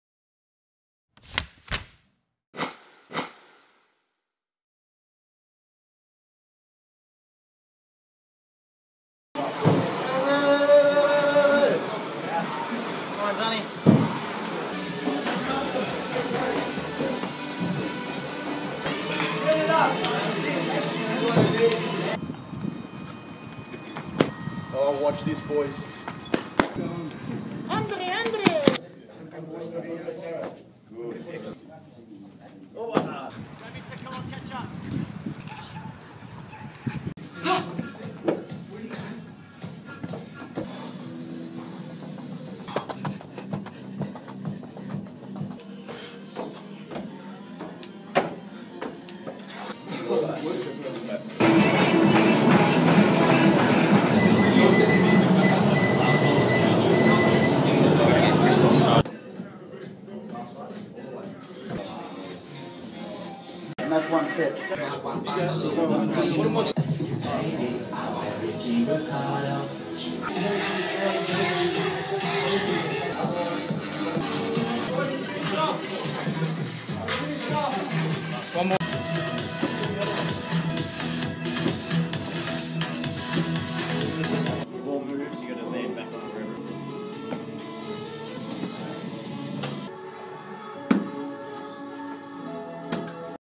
VVFC Strength & Conditioning with Michael Chiovitti
Players going through a S&C program designed by Sport scientist and club trainer Michael Chiovitti. Program focus' on Futsal specific exercises for agility, mobility, explosive power, reaction, core strength as well as high altitude and goal keeper specific trainings.